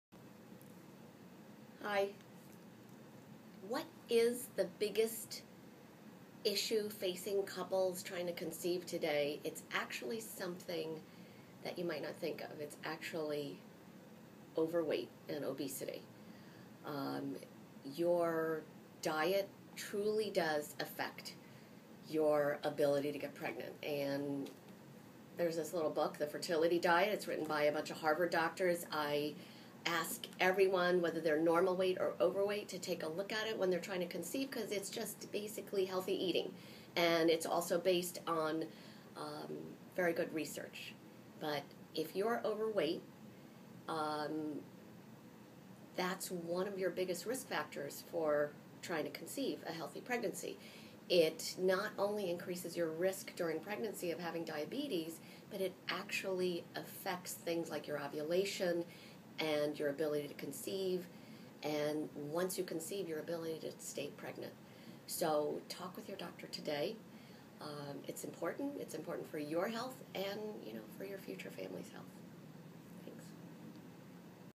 Obesity and Infertility Serena H. Chen, MD
What is one of the biggest risk factors for infertility and pregnancy-obesity.